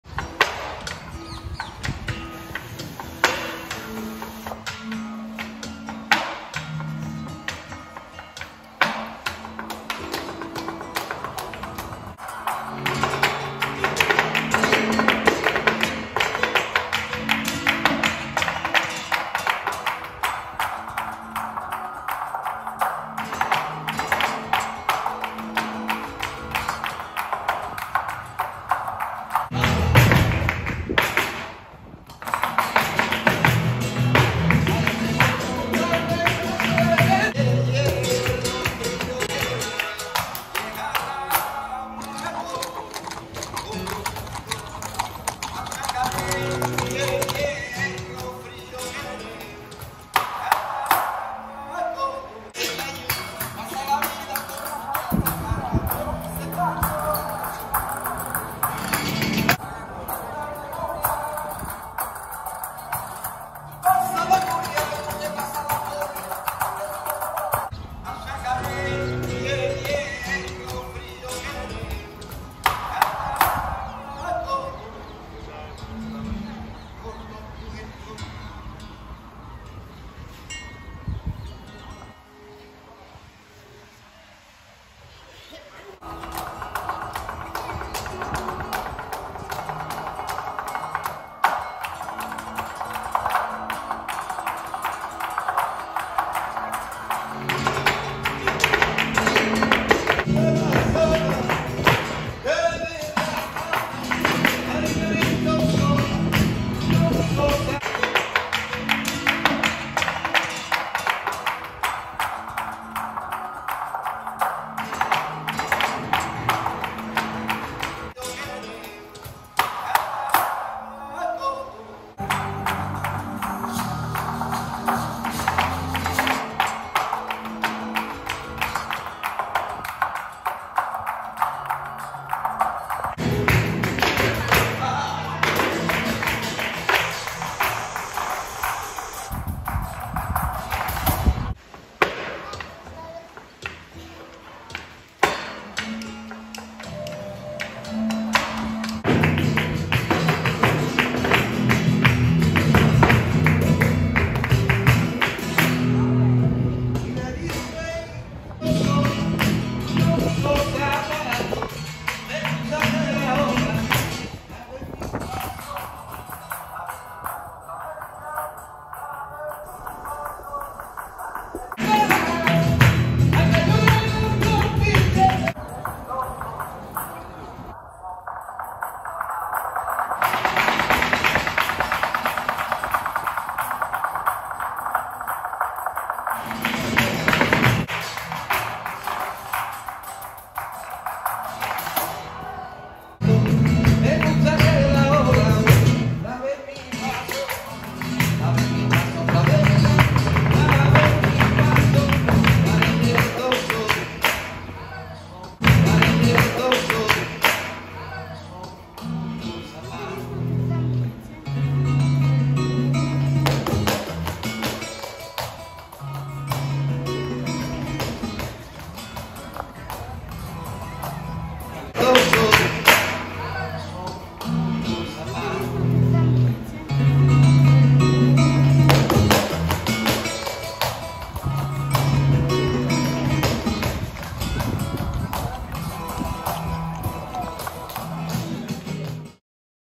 Flamengo at Beautiful Plaza de Espana | april 2023
Flamengo at the  Beautiful Plaza de Espana.
Both filmed with my iPhone in April 2023.

🛠 Tripod:
Manfrotto 504 HD
(amongst many)

⟶Stabilizer:
Ronin RS2

🖥  Edit:
Imac 27 inch scherm
MacBook Pro

📞 Mobile phone:
Iphone Pro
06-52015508